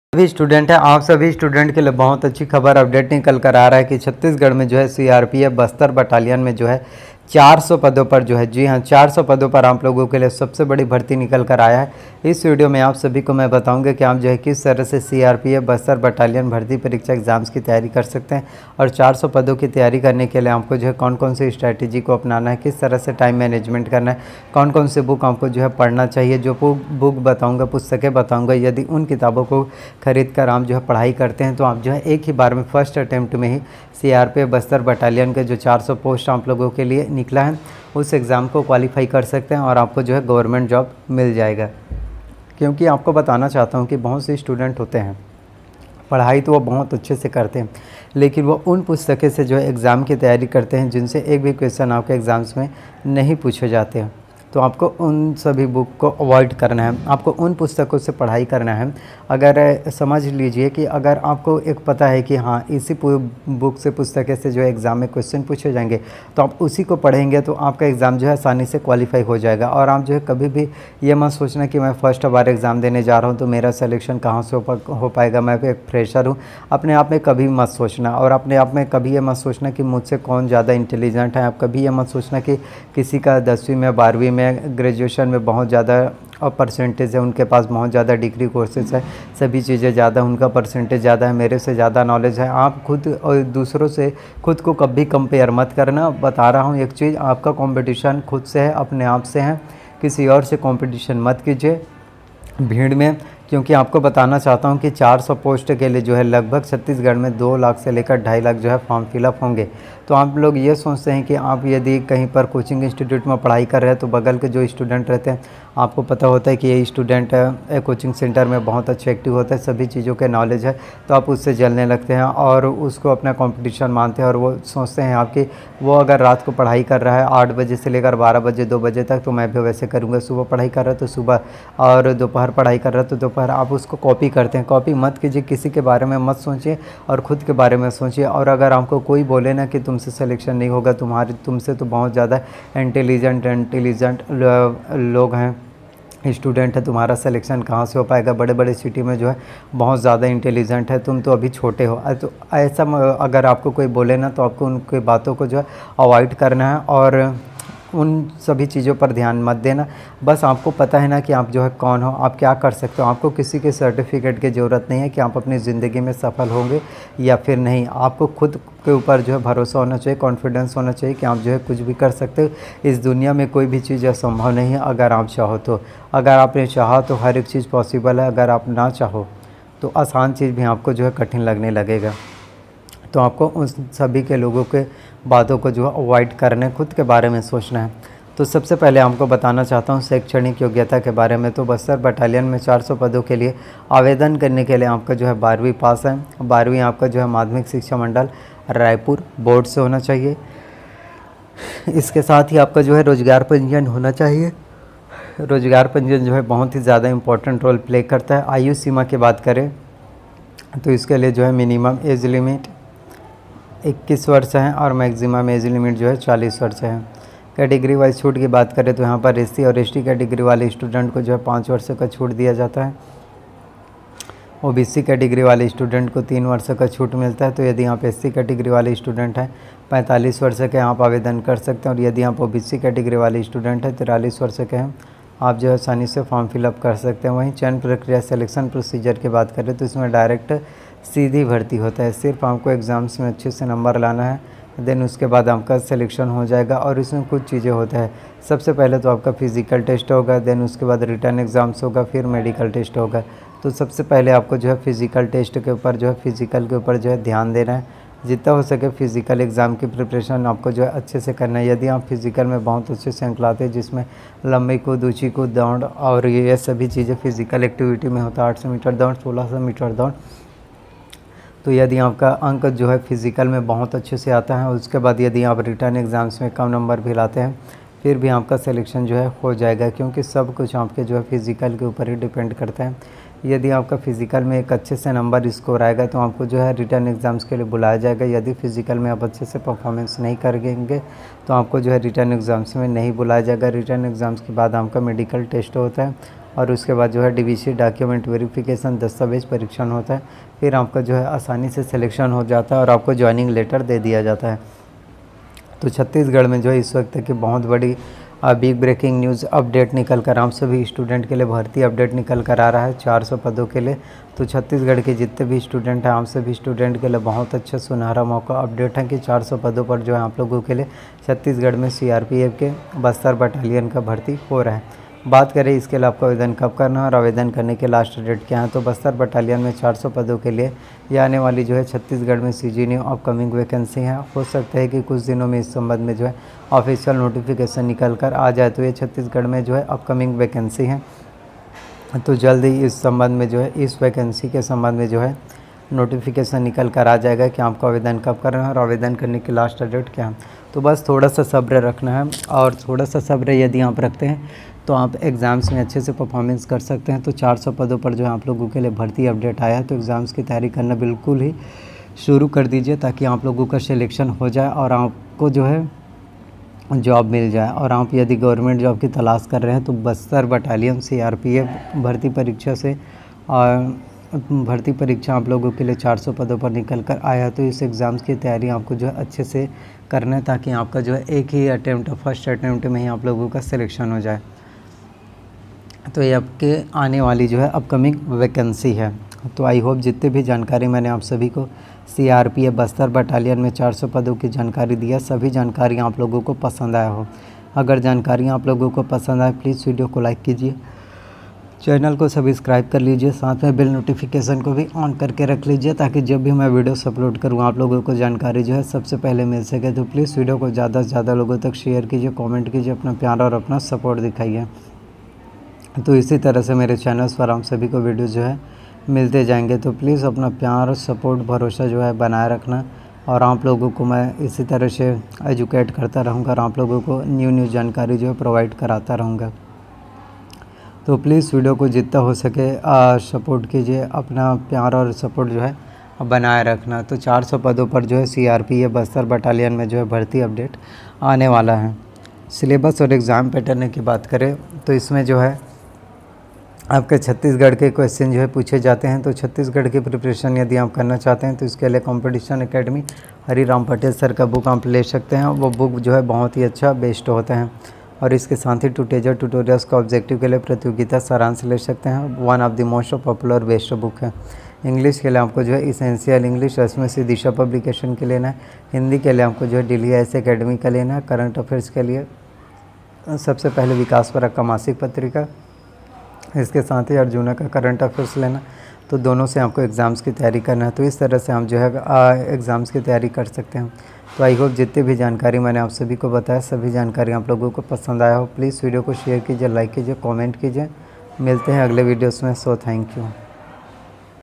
छत्तीसगढ़ में CRPF बस्तर बटालियन के 400 पदों पर सबसे बड़ी भर्ती || cg new vacancy 2022
iss video mein maine bataya hai cg crpf new upcoming vacancy 2022 ke baare mein